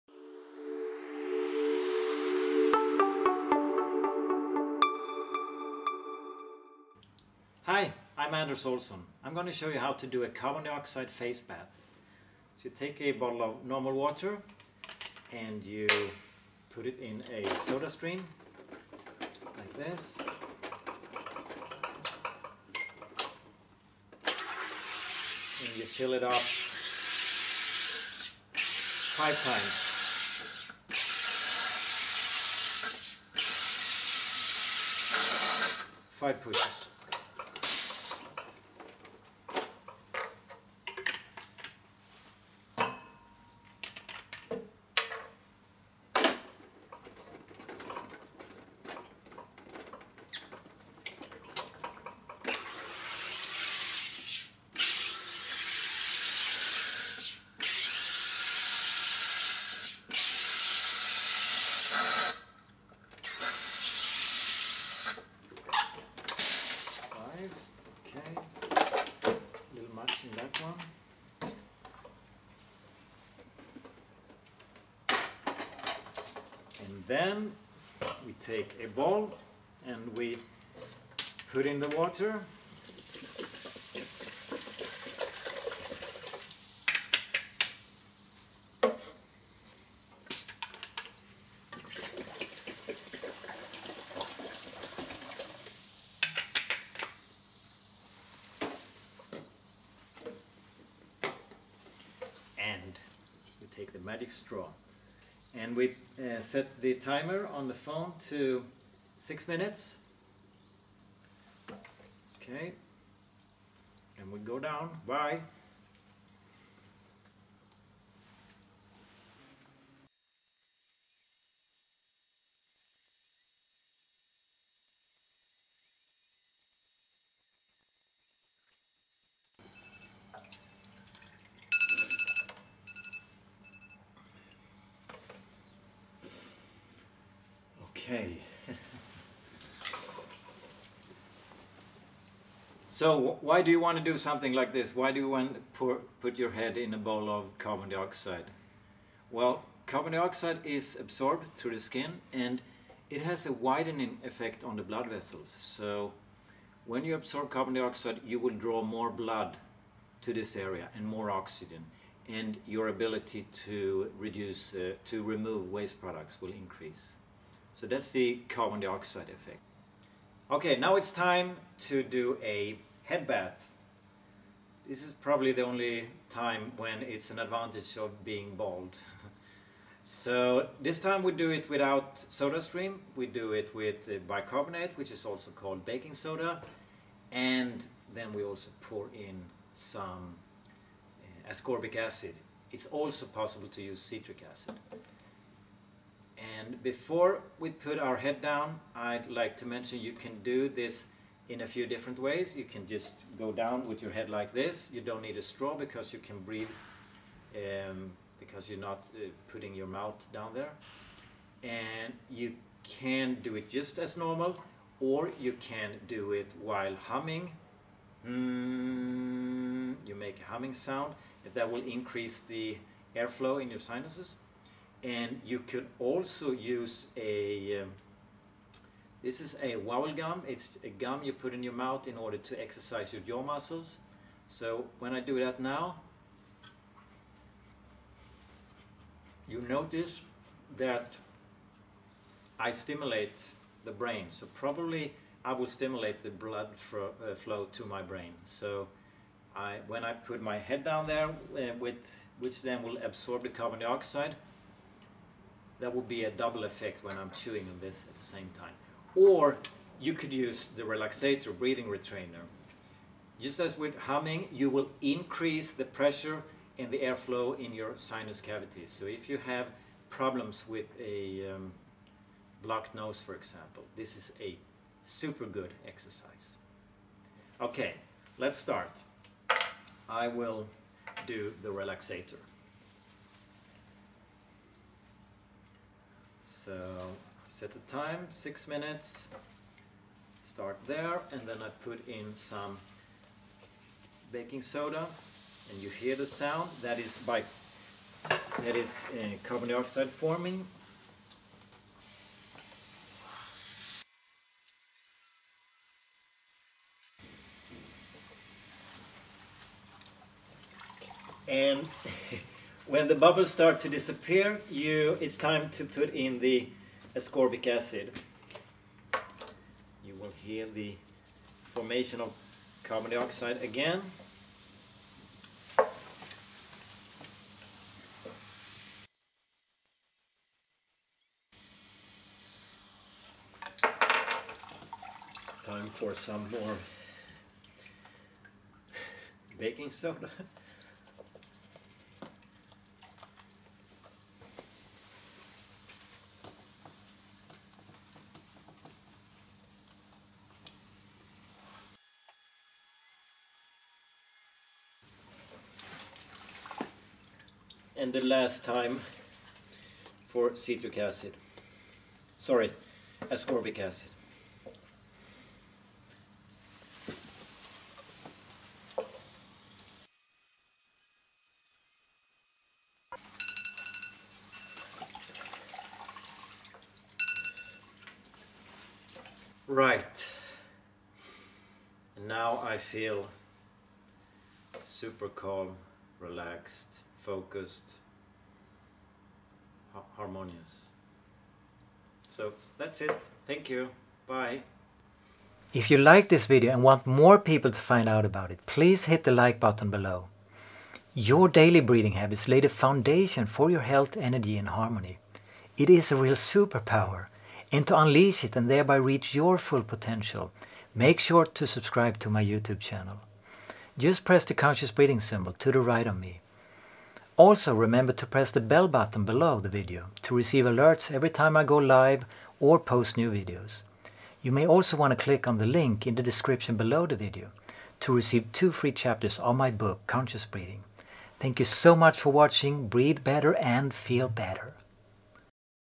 Carbon Dioxide Face and Head Bath
Bathing in hot mineral springs is a well proven way to improve skin problems and poor circulation. It has been found that these springs contain large amounts of carbon dioxide. Artificial mineral springs, where extra carbon dioxide has been added into the ordinary water, have been created to help achieve the same effects as a natural mineral spring. Both natural and artificial carbon dioxide baths are common in Europe and Japan and are used to cure skin problems caused by eczema, psoriasis and severe diabetes.

As the body’s carbon dioxide content rises, the blood vessels are expanded so that the blood circulation is increased. According to a Japanese review of available studies, artificial carbon dioxide baths give effects similar to baths in natural hot springs. Although hot springs also contain many different minerals, the researchers behind the study believe that it is primarily the high carbon dioxide level that gives rise to the positive effects.

This video shows you how to make your own carbon dioxide bath for the face and head.